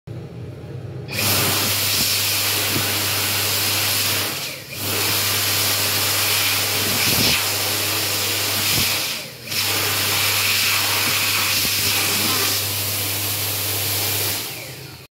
Dyson V hand dryer, Five Guys 🚻, Watermark, Southampton
There a 3 Unisex toilets and this is the one on the right.
This hand dryer is very powerful the sensor wasn't too great. but it does dry my hands.